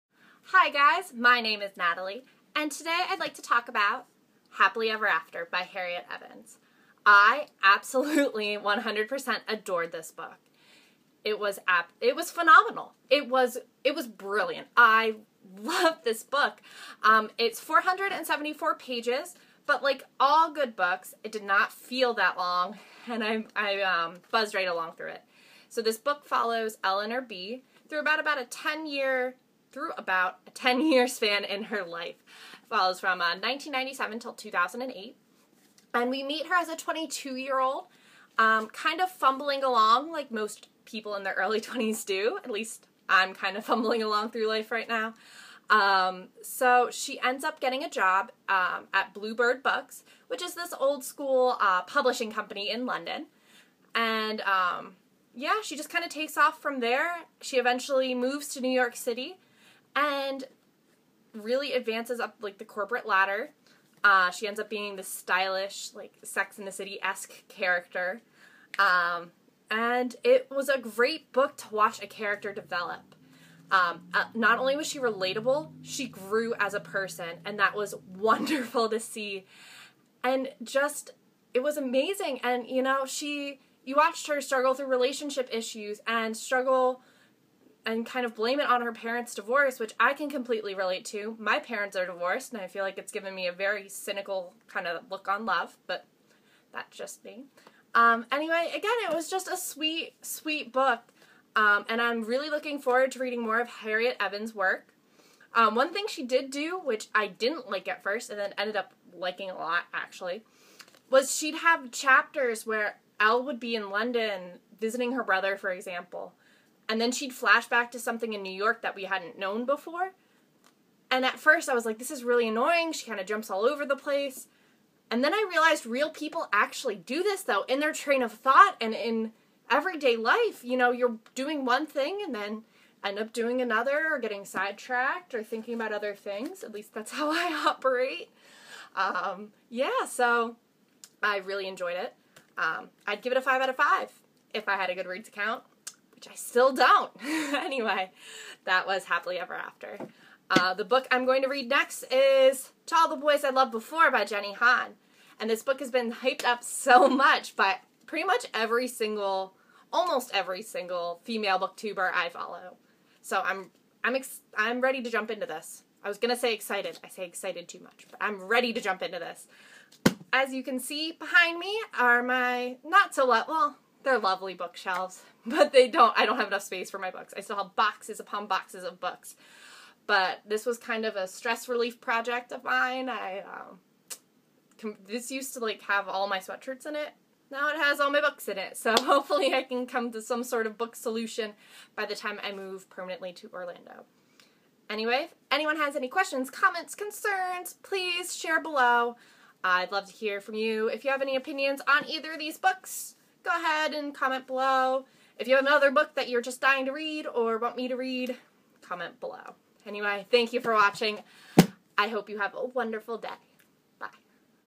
Book Review: Happily Ever After
My thoughts on Happily Ever After by Harriet Evans